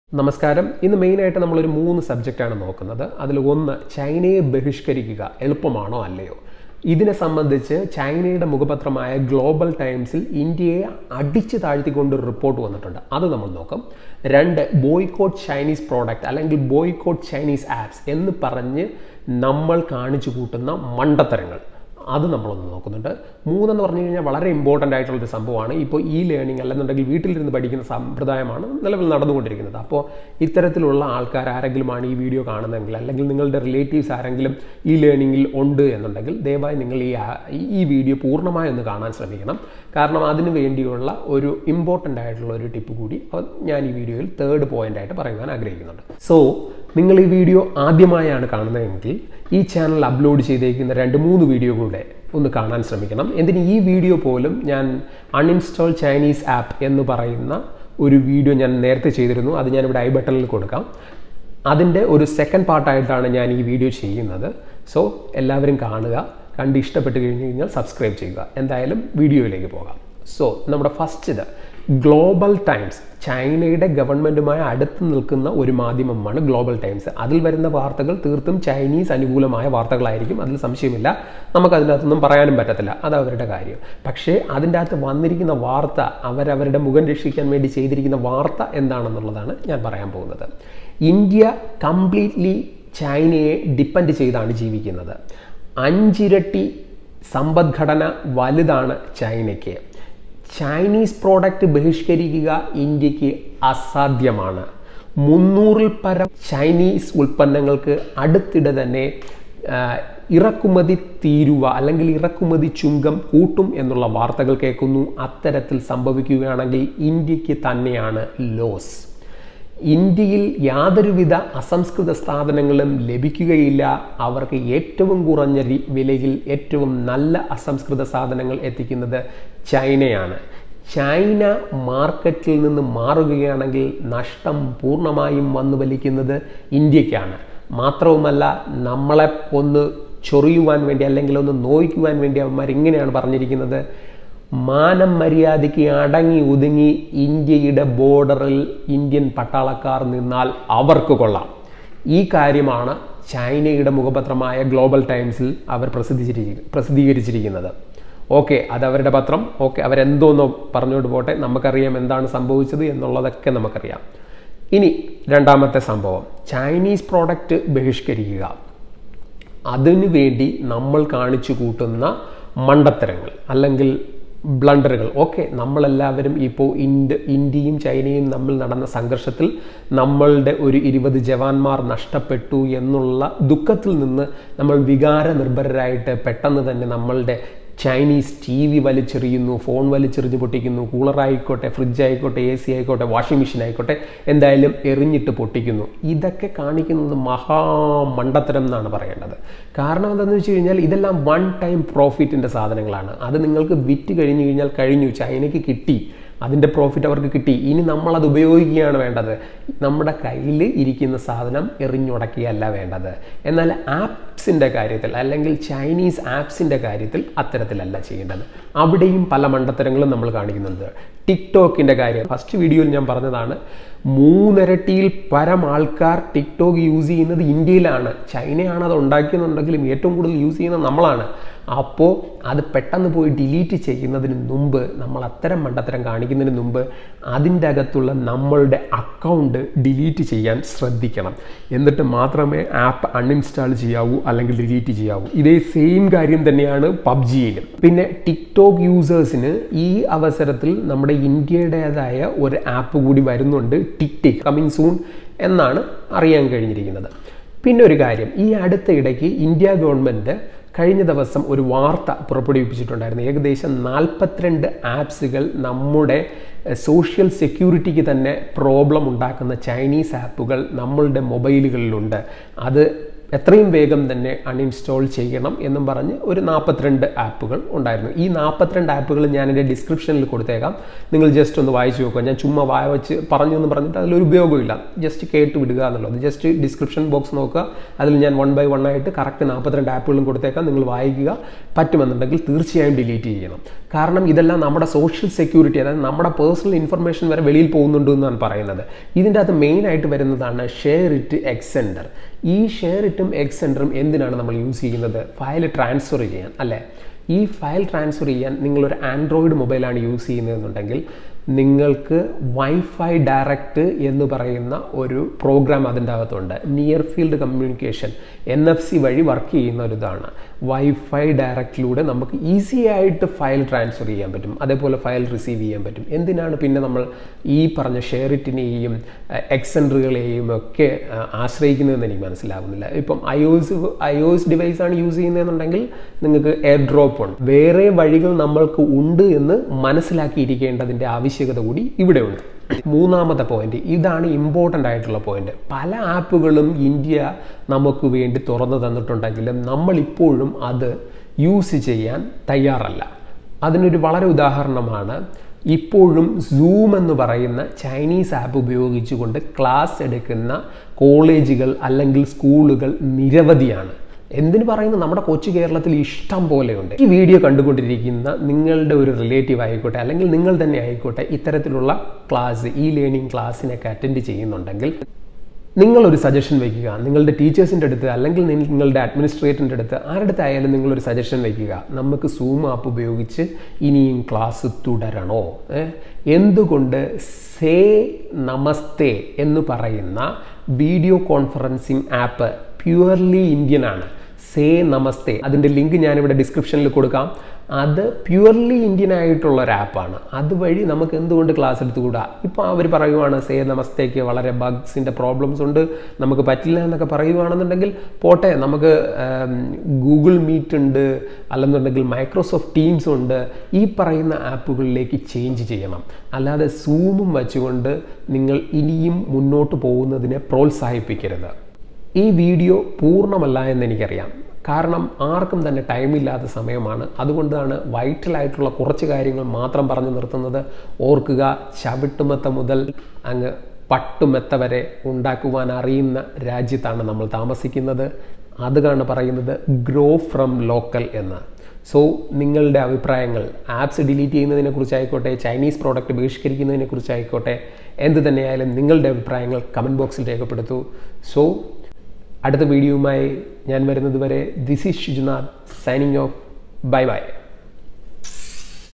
Mistakes in boycotting China Apps and productsPart2|Malayalam
Avoid or boycottchinese products are possible or not
Government reportedly lists 42 Chinese apps as dangerous,
Here are the apps that have been listed, according to the report:
,WeChat,
 SHAREit,
 UC News,
 UC Browser,
 BeautyPlus,
 NewsDog,
 VivaVideo-
QU Video Inc,
 Parallel Space
, APUS Browser,
 Perfect Corp,
 Virus Cleaner
(Hi Security Lab),
 CM Browser,
 Mi Community,
 DU recorder,
 Vault-Hide,
 YouCam Makeup,
Mi Store,
CacheClear
 DU apps studio,
 DU Battery Saver,
 DU Cleaner,
 DU Privacy,
360 Security,
DU Browser,
Clean Master
 Cheetah Mobile
, Baidu Translate,
Baidu Map,
Wonder Camera,
ES File Explorer,
 Photo Wonder,
 QQ International,
QQ Music,
QQ Mail,
 QQ Player,
 QQ NewsFeed,
WeSync,
QQ Security Centre
, SelfieCity,
Mail Master,
Mi Video call-Xiaomi,
 and QQ Launcher.

Namaste!

Please use the following link to download the Say Namaste Video Conferencing app on your phone: